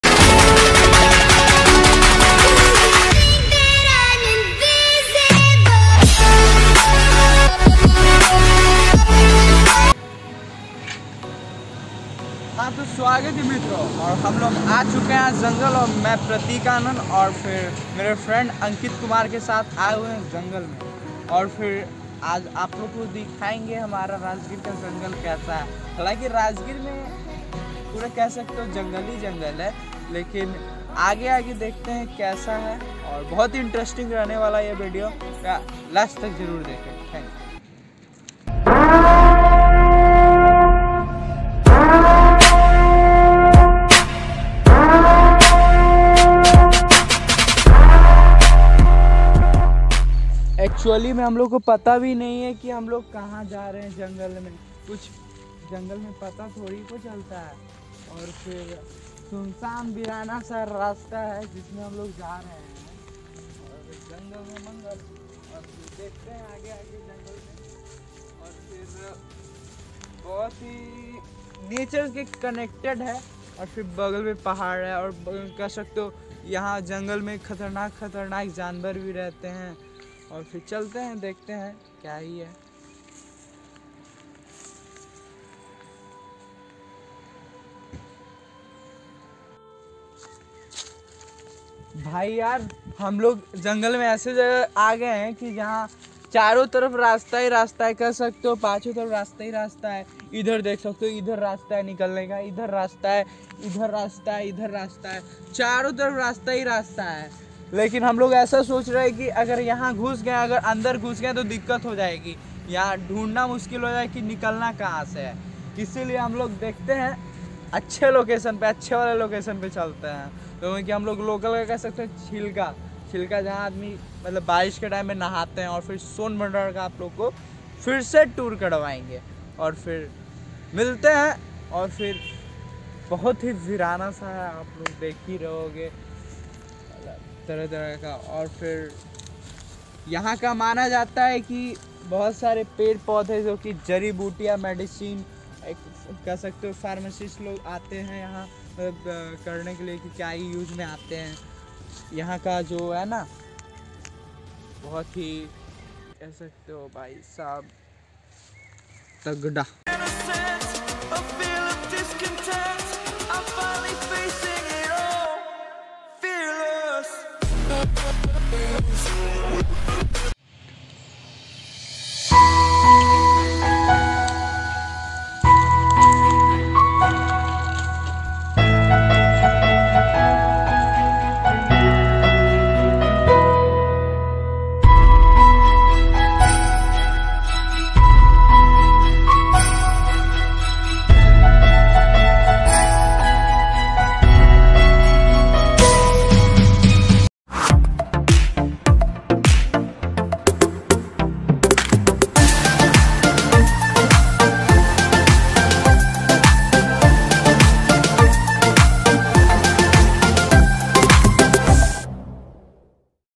JUNGLE Vlog 😱 OH MY GOD Haunted 🥵 'DO OR DIE " rajgir vlog rajgirjungle Jungle Vlog in hindi
Hello friends
I am Pratik Anand and today I was going to jungle for vlog 😅😅
Jungle me mangal first time
my first vlog in jungle
my first vlog in forest
Haunted
maja aagya